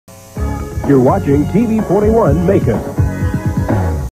WMGT Station ID 1992-93
This is WMGT's station ID from NBC's 1992-93 season "It's A Whole New NBC"
WMGT is located in Macon, GA